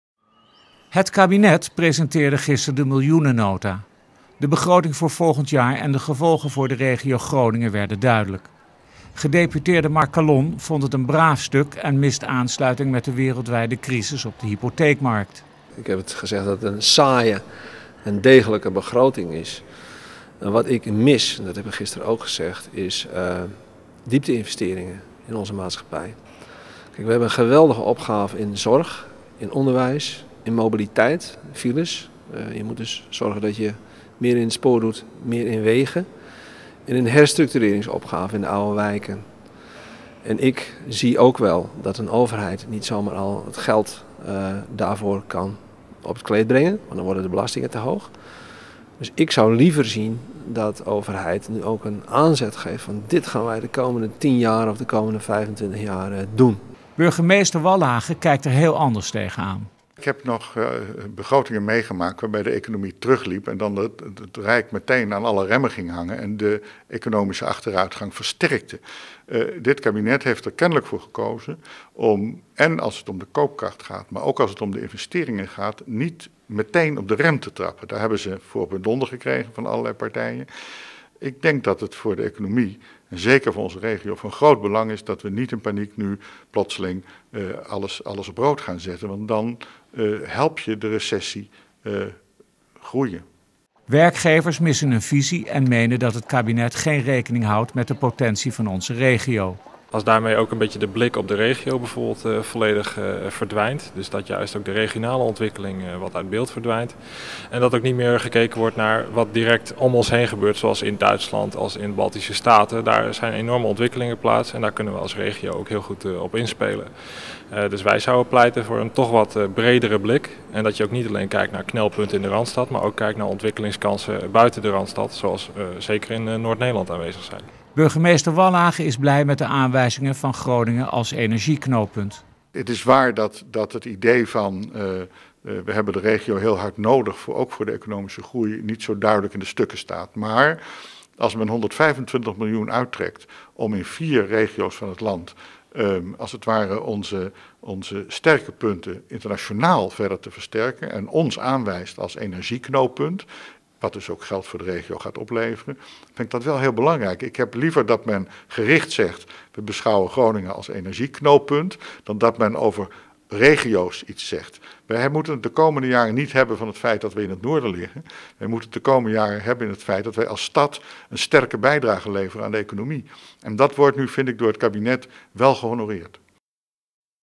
Te weinig oog voor het noorden in de miljoenennota.
OOG TV Nieuws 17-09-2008 - Te weinig oog voor het noorden in de miljoenennota.